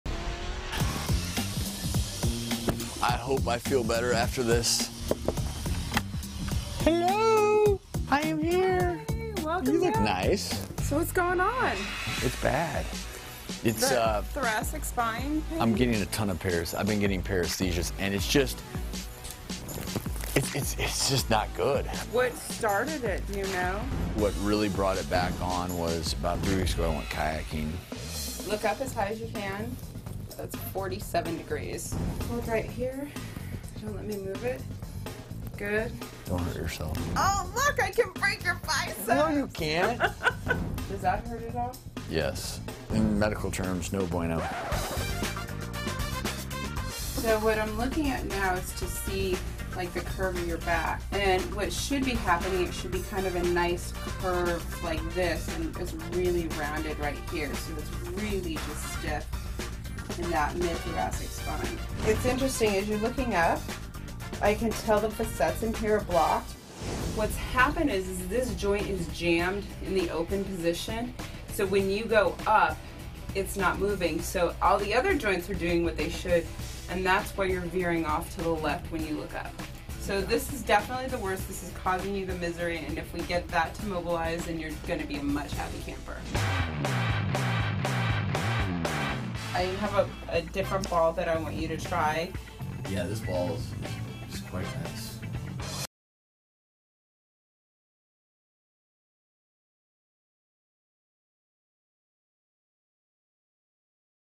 Dr. Travis Undergoes Physical Therapy for Back Pain
ER physician Dr. Travis Stork visits Body Connection in Los Angeles. Watch as he undergoes physical therapy with Danielle to help alleviate back pain.